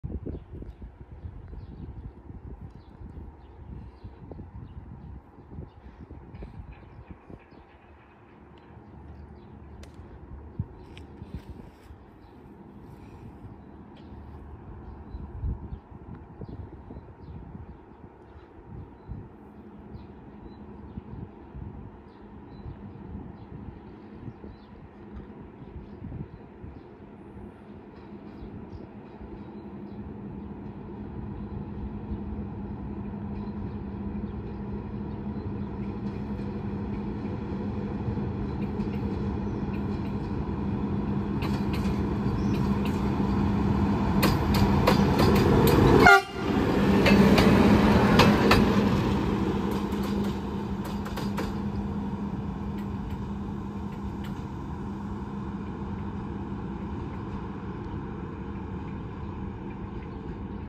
86 005 като ИзЛ 40996 и поздрав / 86 005 as IL 40996 and greeting
86 005 като Изолиран Локомотив 40996 (Долапите - Русе Разпределителна), сниман на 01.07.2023.
86 005 as Isolated Locomotive 40996 (Dolapite - Ruse Razpredelitelna), photographed on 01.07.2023.

Приятно гледане!